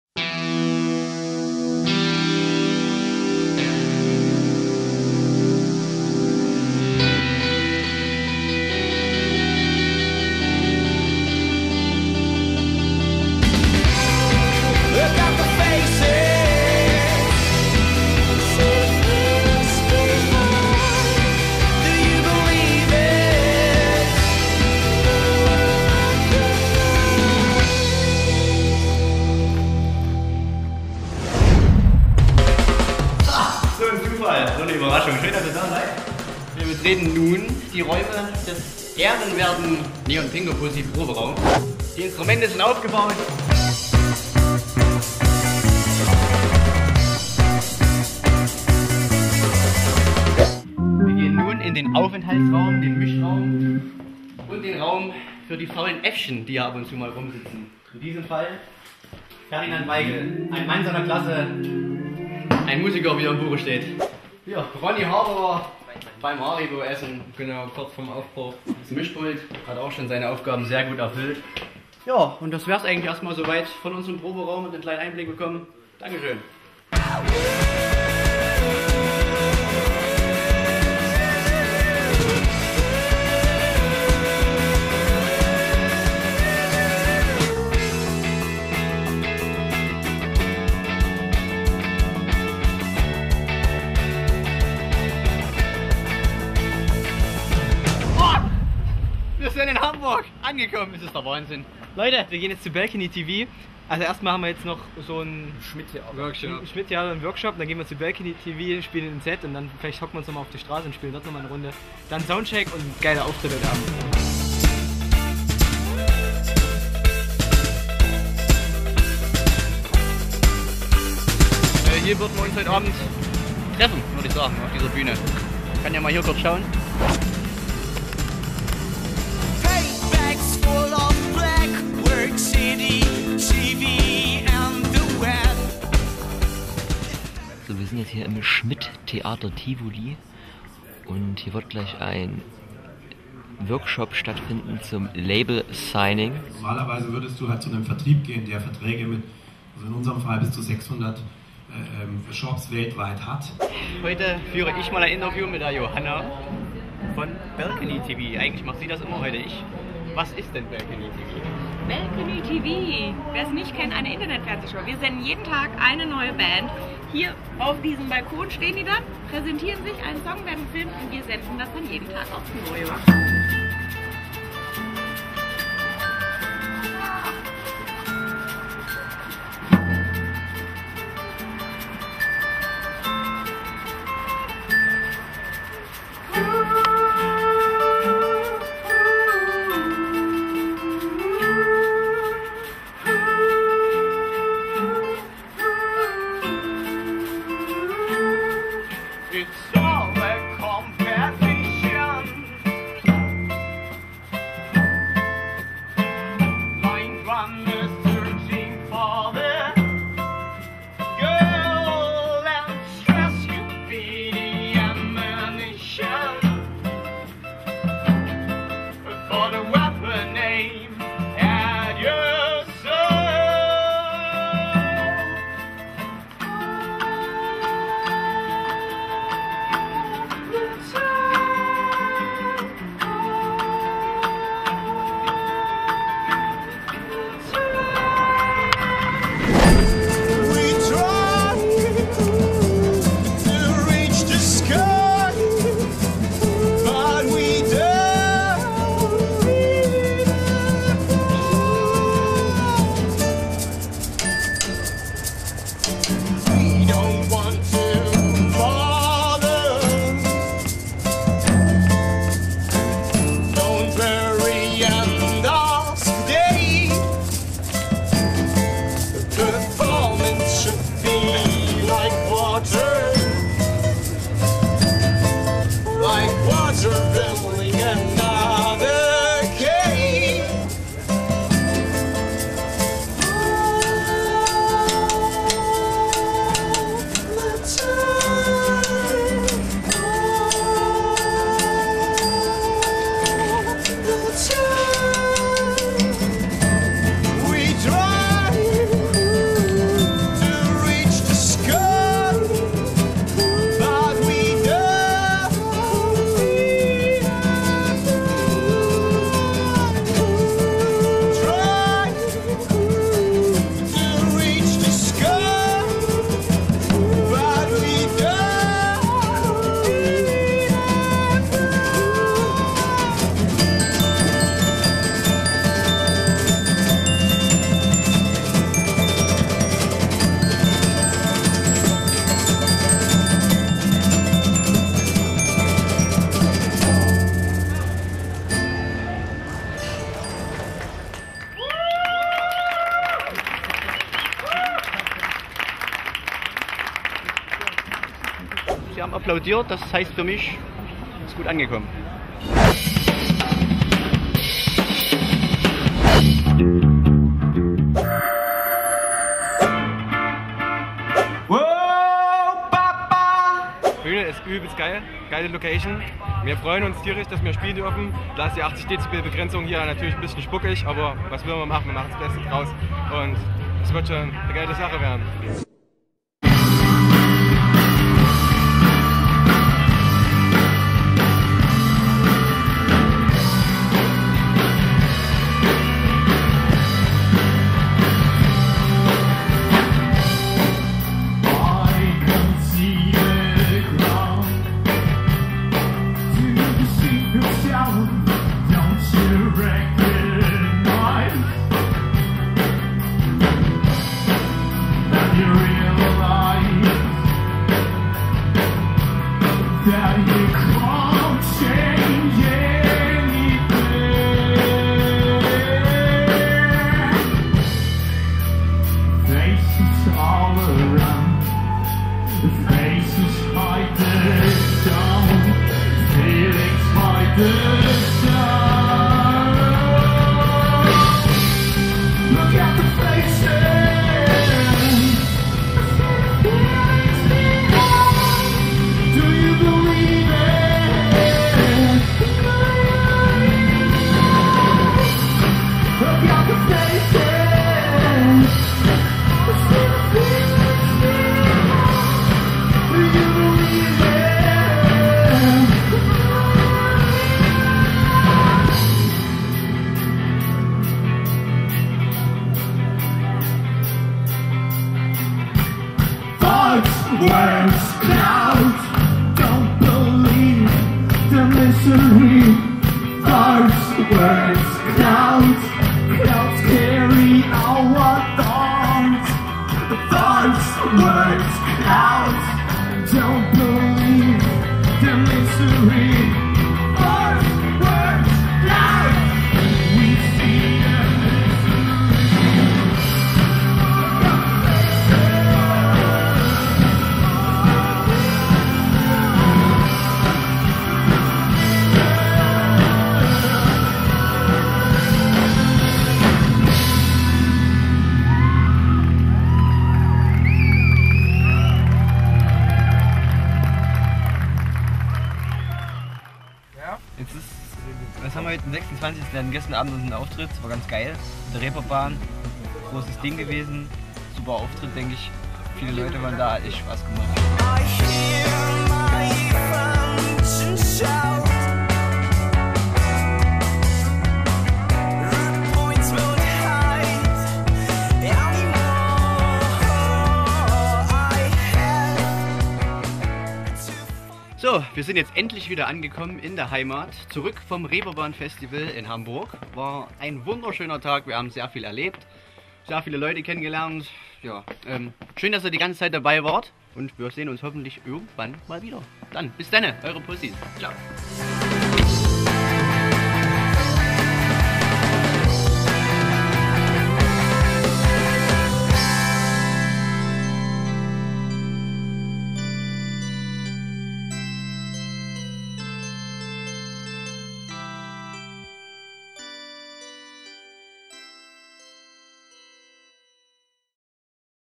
Neon Pingu Pussys goes Reeperbahn - Dokumentation
Begleitet die Band "Neon Pingu Pussys" auf Ihrem Trip nach Hamburg. Werdet Teil Ihrer verstärkten und unverstärkten Songs - live performt. Eine Dokumentation von fünf Musikanten; zwischen Bordstein und Schwalbe machten sie sich die Reeperbahn zu Eigen. Öffnet Augen, spitzet Ohren. Neon Pingu Pussys goes Reeperbahn - nicht in 3D.